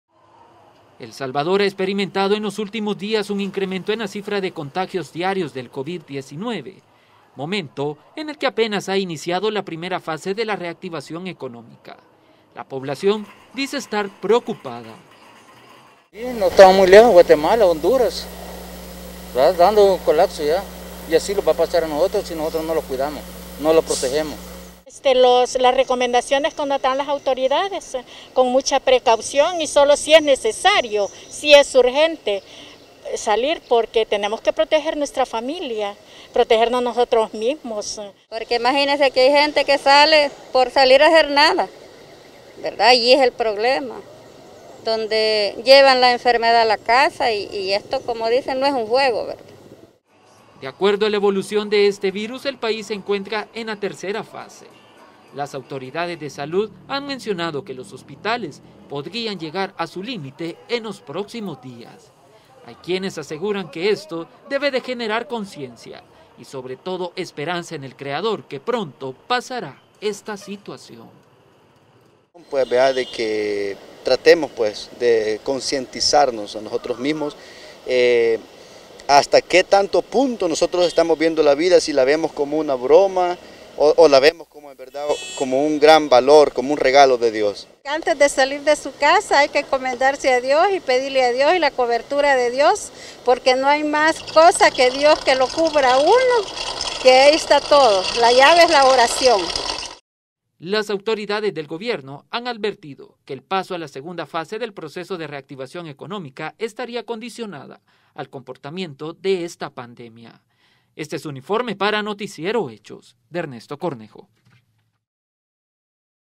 Salvadoreños preocupados por el incremento de casos de covid 19
Luego de la reapertura económica en el Salvador los casos van en aumento, hecho que mantiene con preocupación a la población salvadoreña.

¡Visítenos!